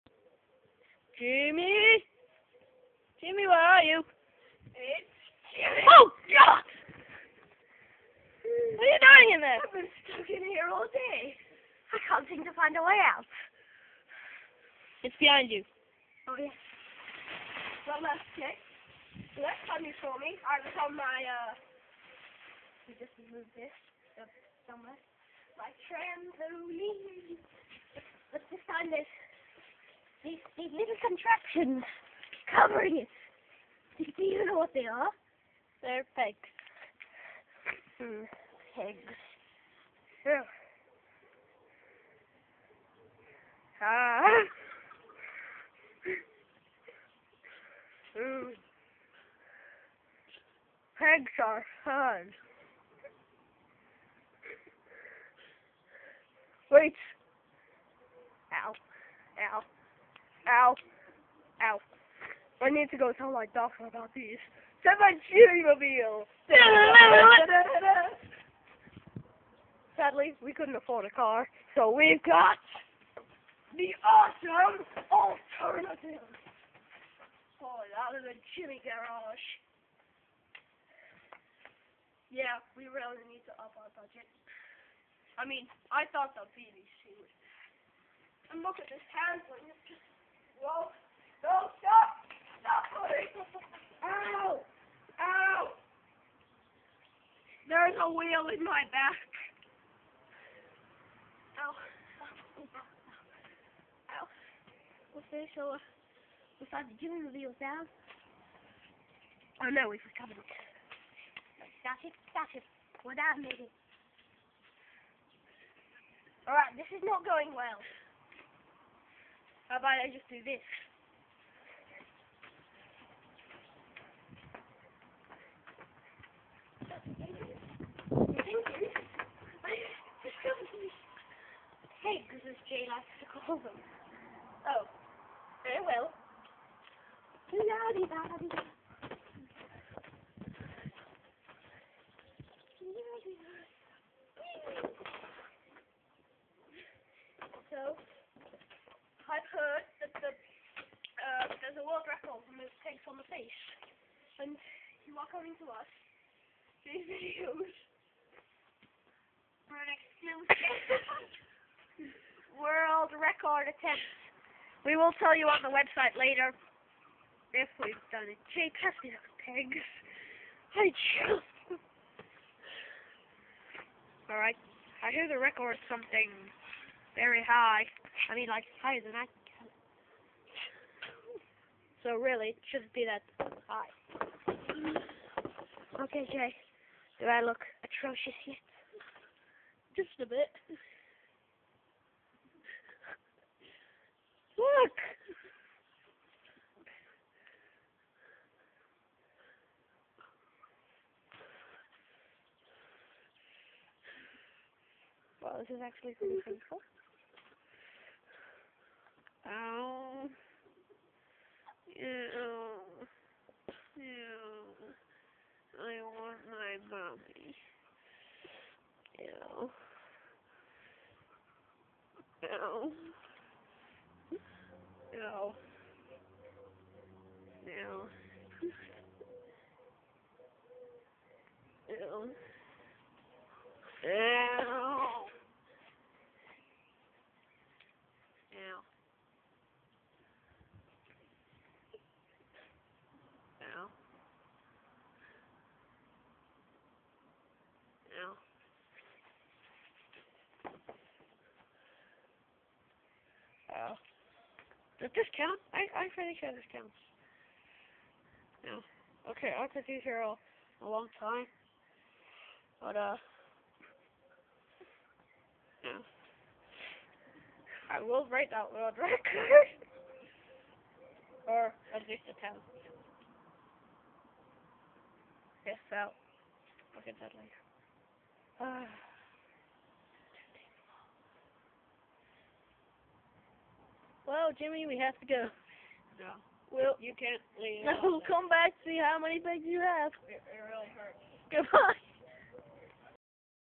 Jimmy trys to break a world record
see if Jimmy can break a world record for most pegs on face